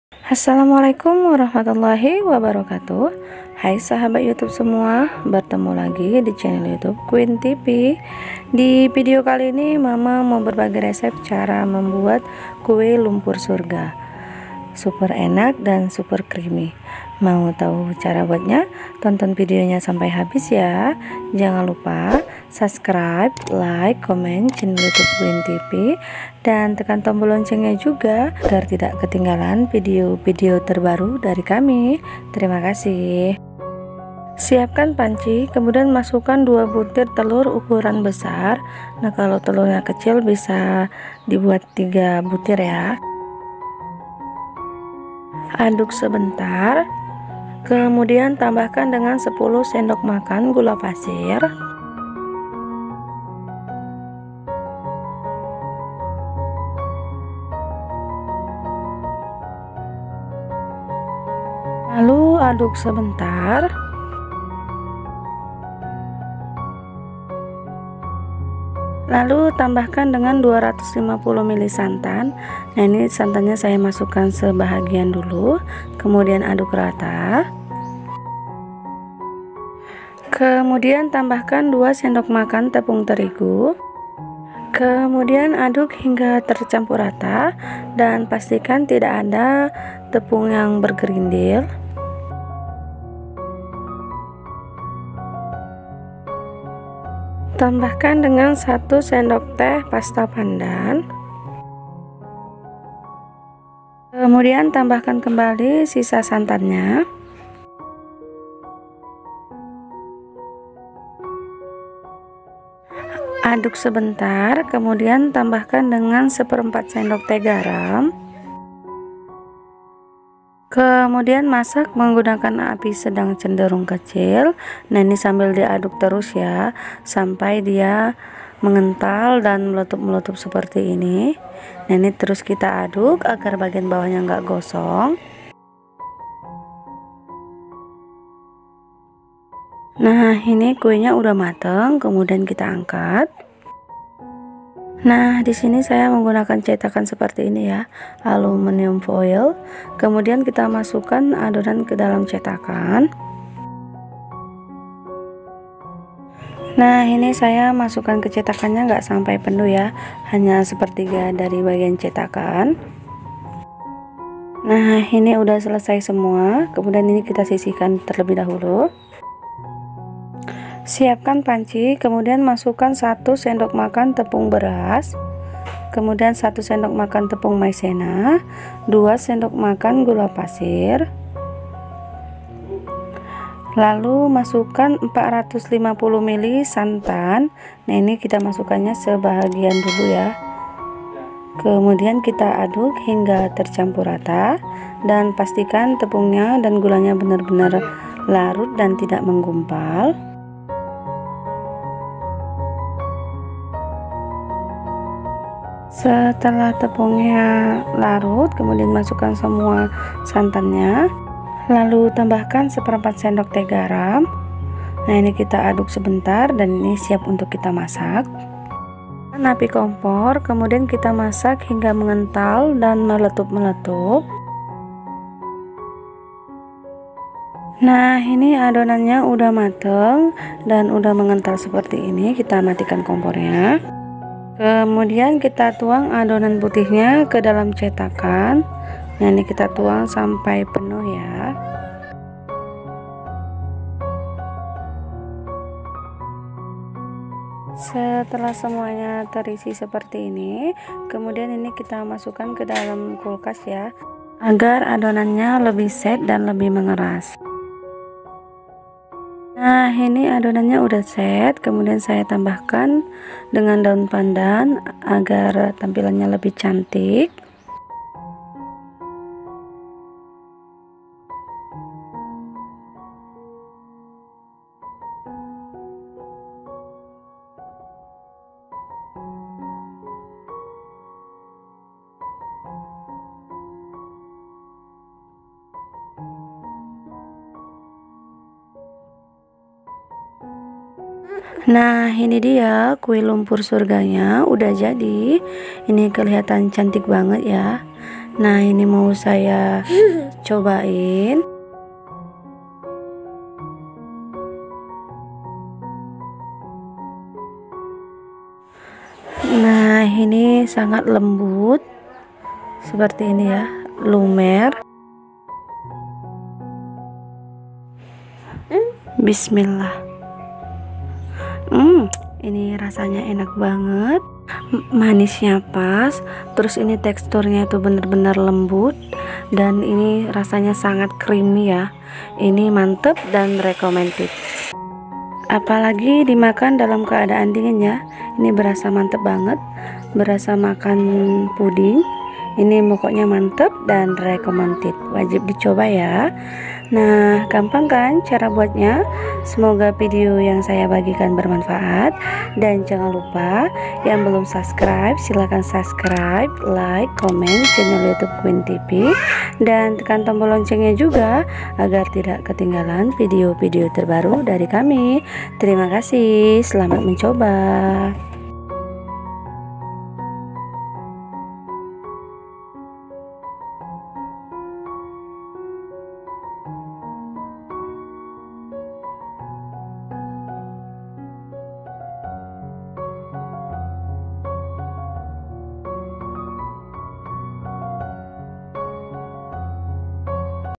HANYA DI ADUK JADI KUE LUMPUR SURGA SUPER ENAK, LEMBUT DAN CREAMY || TANPA KUKUSAN
Bahan A
2 butir telur ukuran besar
10sdm gula pasir
2sdm tepung terigu
250-260ml santan
pasta pandan secukupnya
garam secukupnya

Bahan B
450ml santan
2sdm gula pasir
1sdm tepung beras
1sdm tepung maizena
garam secukupnya

Selamat datang di CHANNEL kami, Semoga Video Quin TV Dapat Menghibur Dan Bermanfaat untuk teman teman semua.